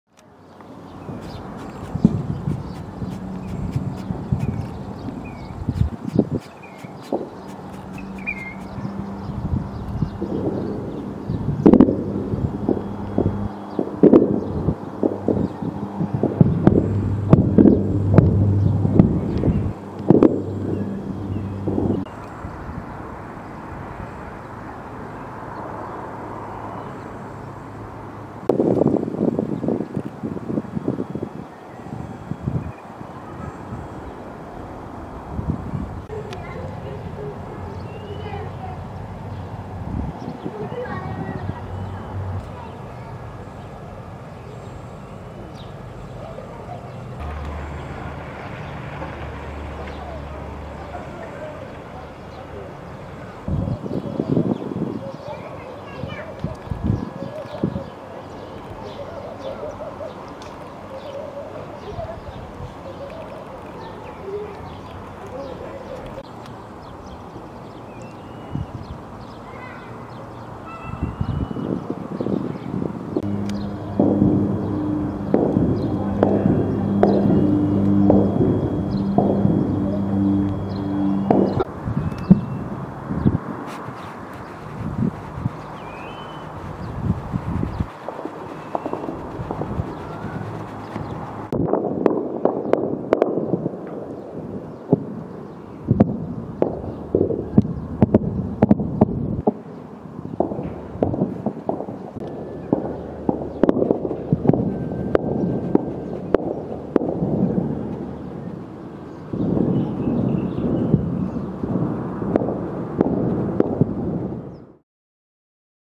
Images of smoke, reconnaissance aircraft flying over Khartoum | AFP
Images of smoke and a reconnaissance aircraft flying over Khartoum, as deadly battles between the army and paramilitary forces led by rival generals and explosions rock the Sudanese capital for a fifth day in a row.